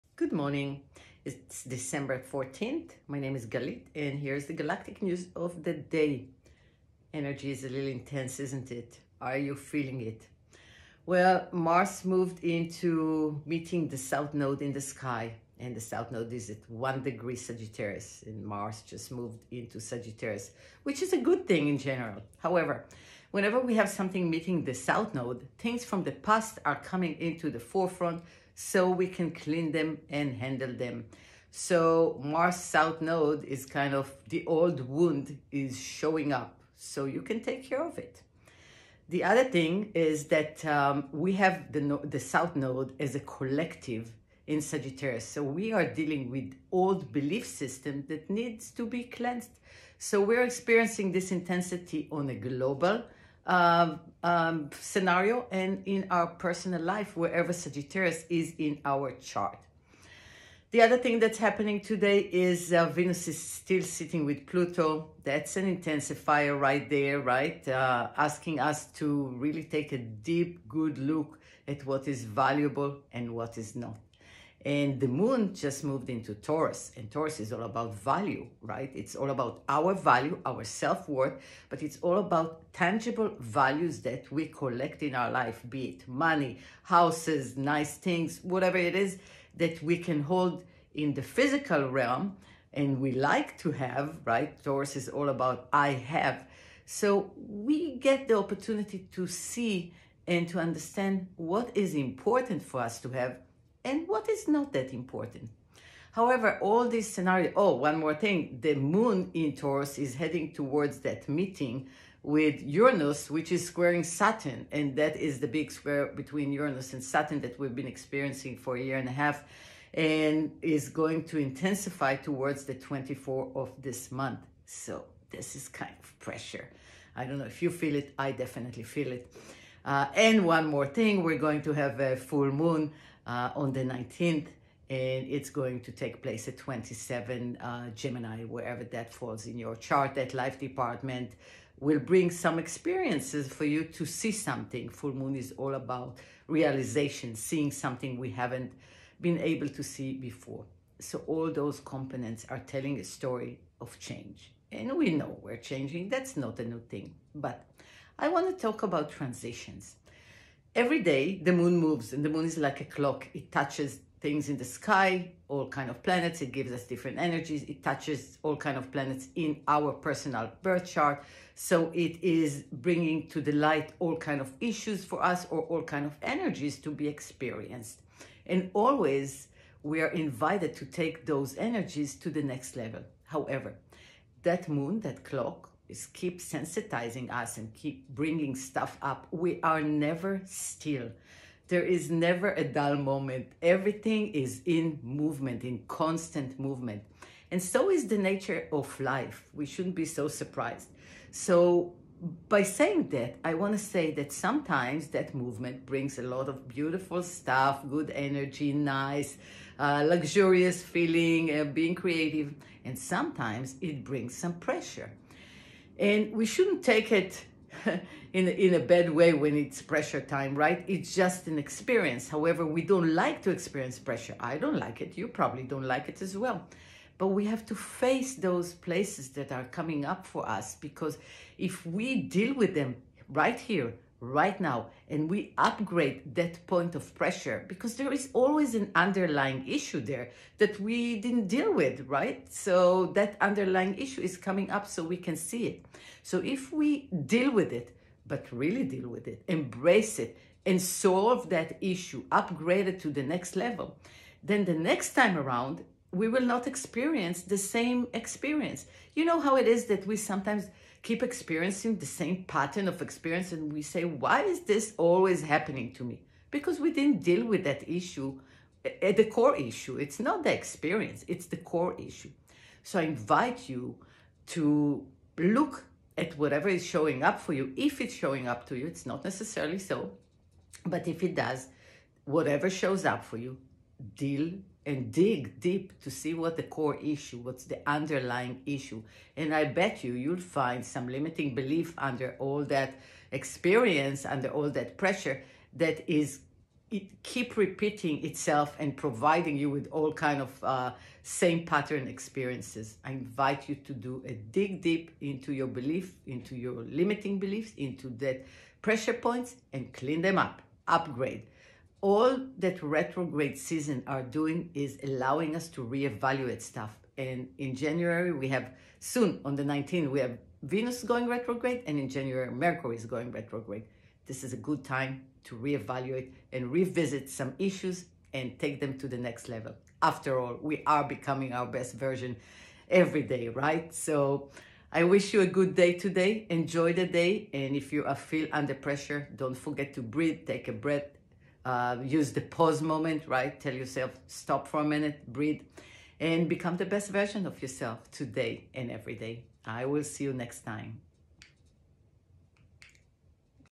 Are you experiencing an old limiting belief showing up again?
Mars conjunct the south node in Sagittarius brings old limiting beliefs that we can become aware of and deal with a core Issue.
What is coming up for you now?